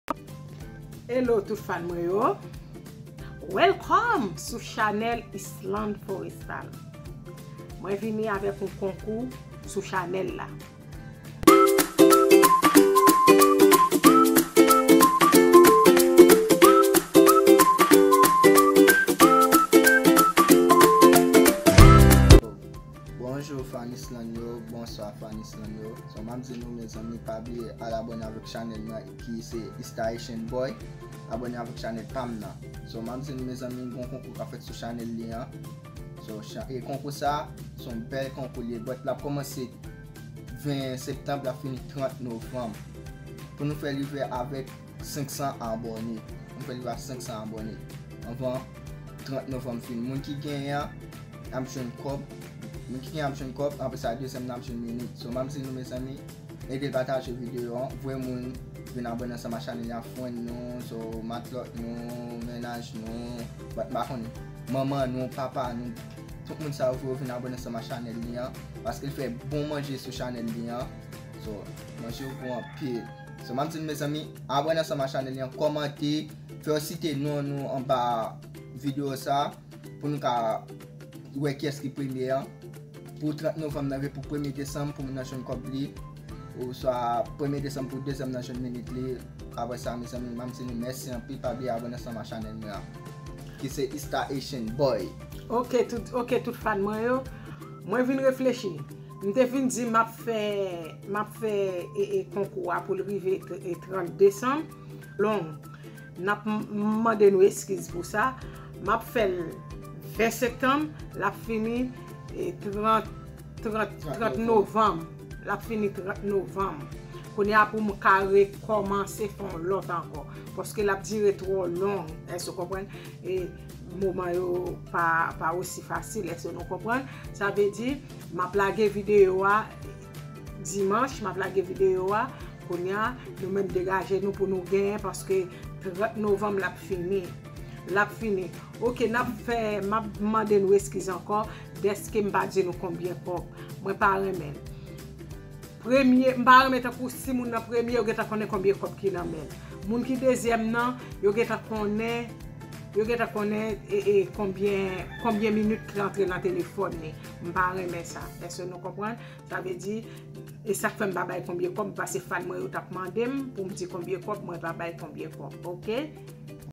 GIVEAWAY Fem Rive a 500 Subscriber
mezanmi  yon kankou  mw balance  sou channel lan fem rive a 500 subscriber     anvan 30 Nov  but nap chwazi de moun ki voye plus  moun yo
1 er  an ap jwen kob
2 em lan ap jwen munit
 merci anpil zanmi yo mw konte sou  Nou